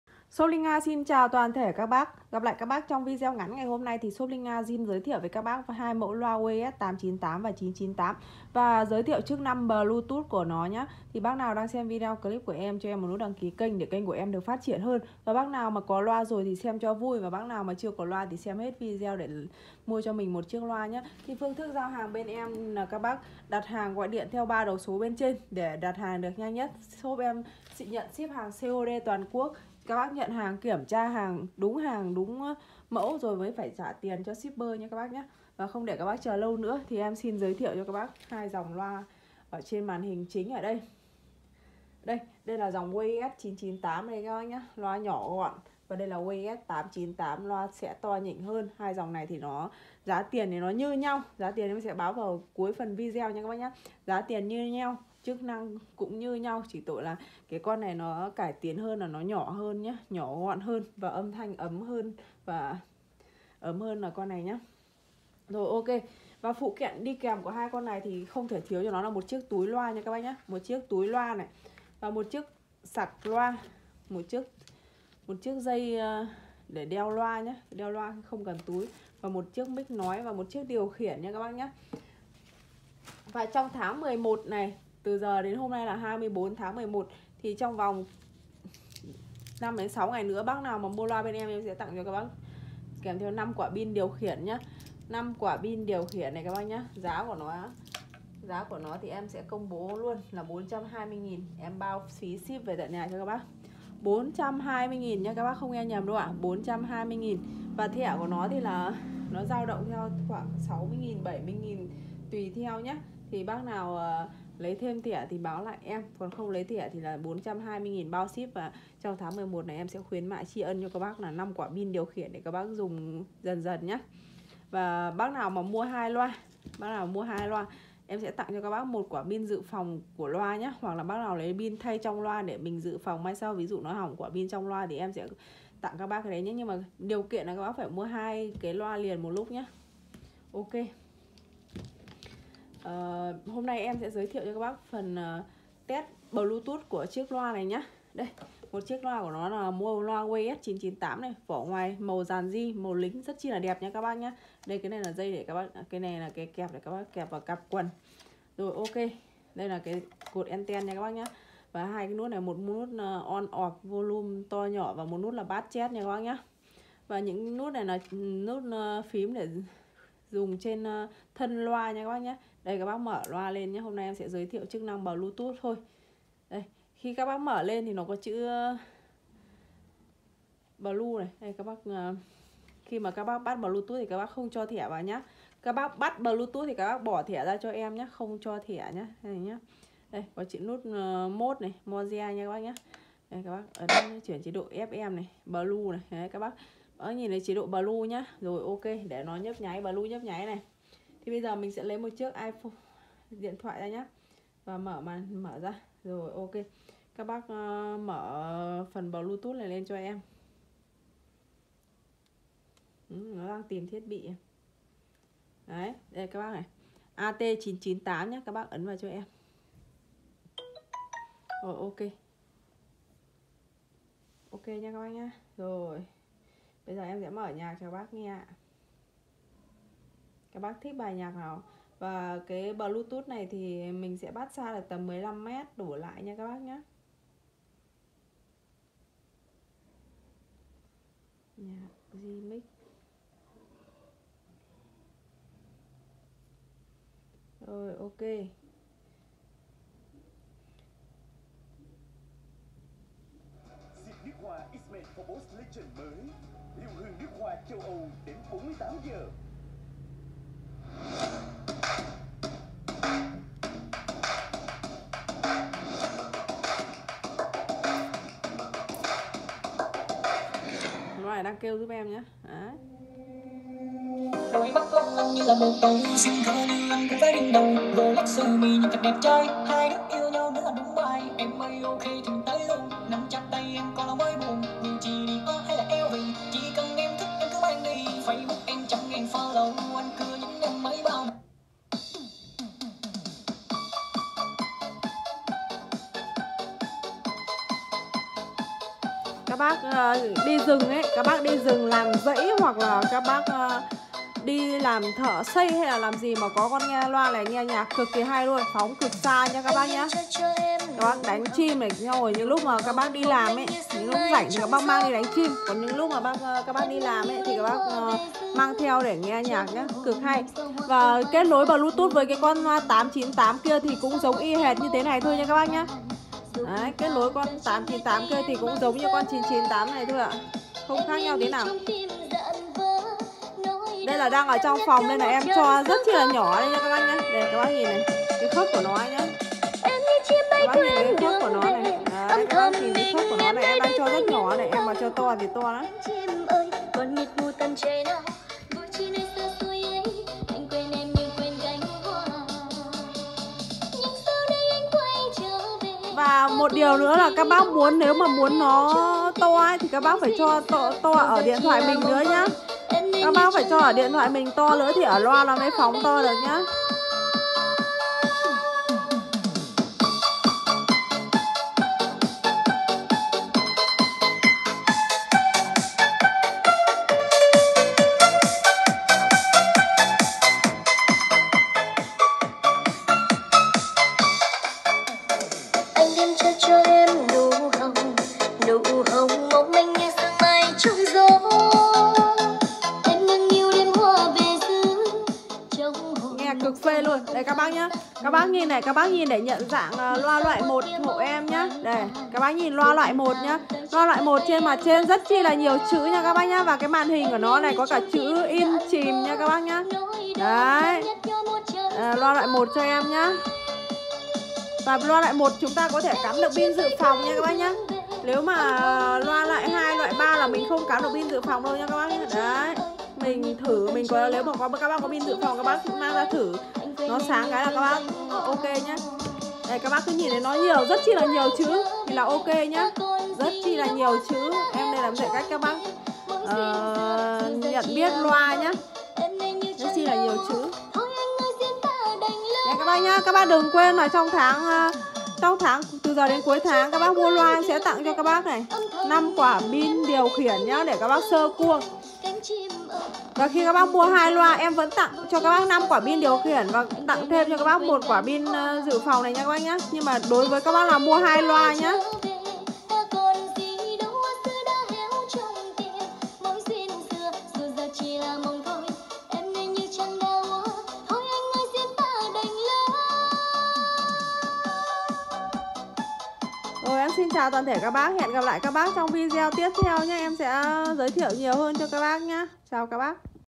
Loa 898 998 bẫy gà rừng cực ăn 👉có Bluetooth trong loa luôn ☎️0962883207
Loa ws898 ws998 bẫy gà rừng các loại chim … hướng dẫn bắt Bluetooth ☎️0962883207